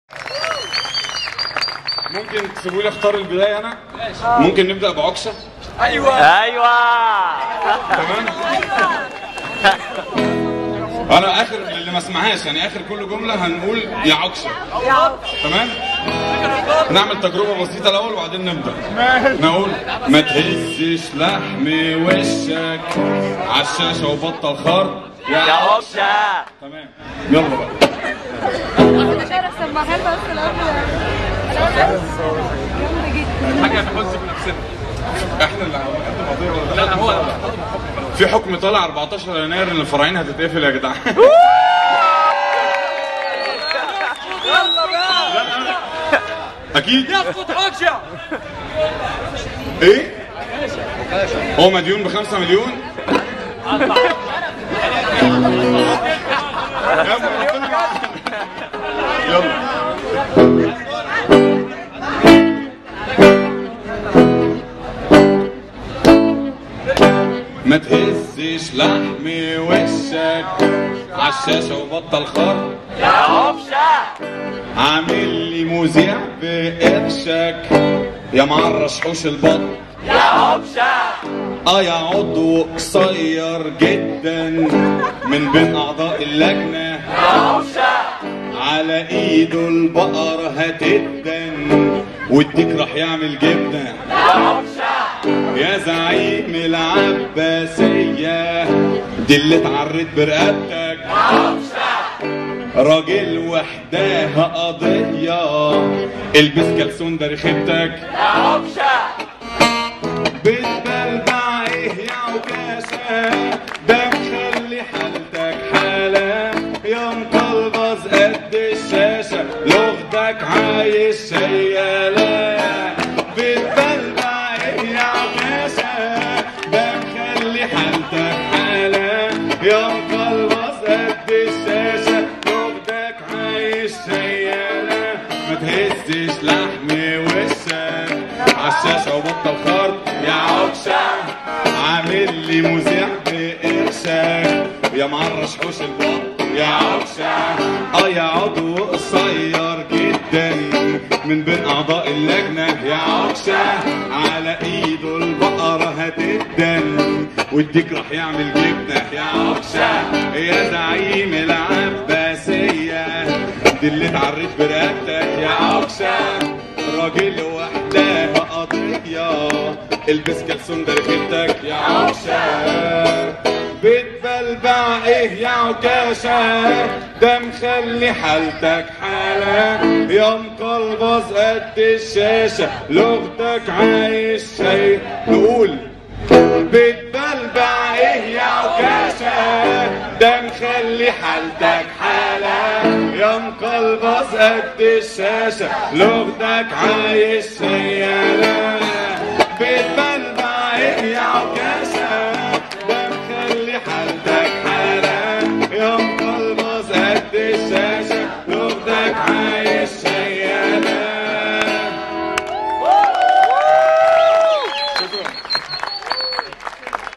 رامي عصام يرسل التحية لتوفيق عكاشة
Ramy Essam mocking Tawfiq Okasha, the counterrevolutionary sensationalist TV figure.
Qasr el-Nil Street, Downtown Cairo.